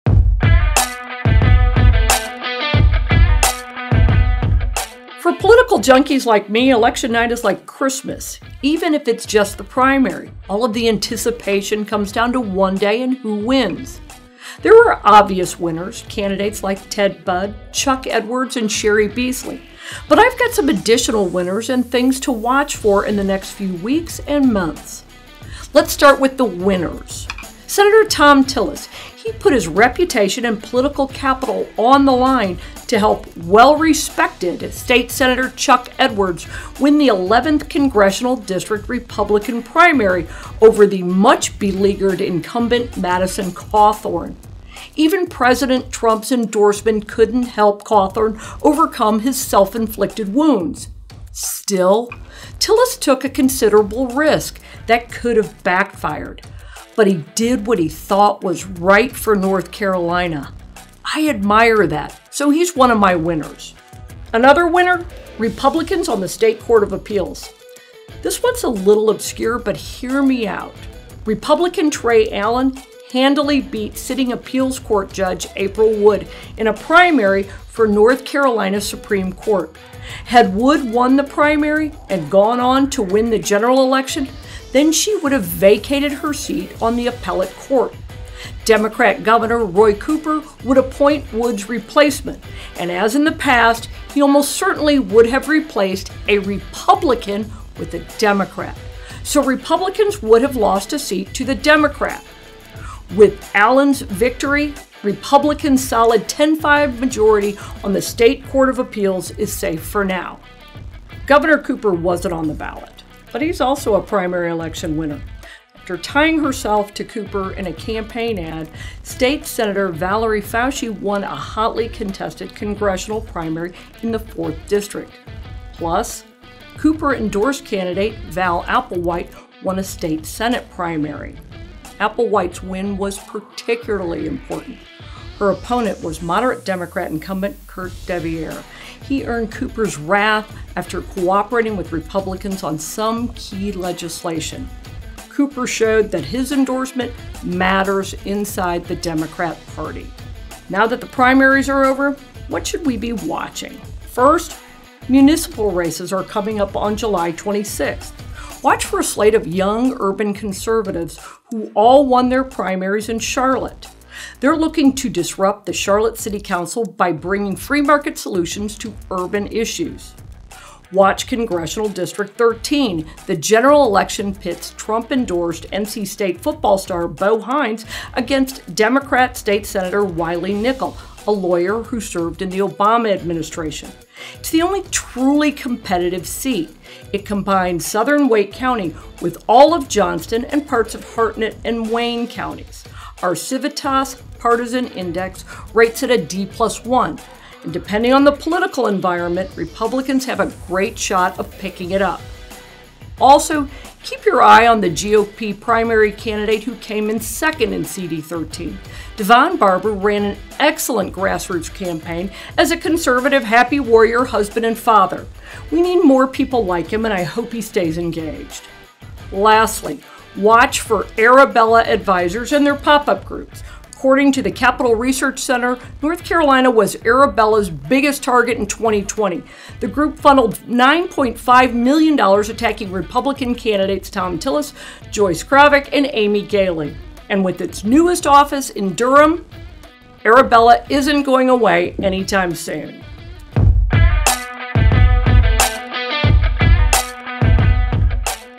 The Primaries are Over - Now What?
For political junkies like me, election night is like Christmas – even if it’s just the primary. All the anticipation comes down to one day and who wins. Now that the primaries are over, what should we be watching? There were obvious winners. But I’ve got some additional winners and things to watch for in the next few weeks and months.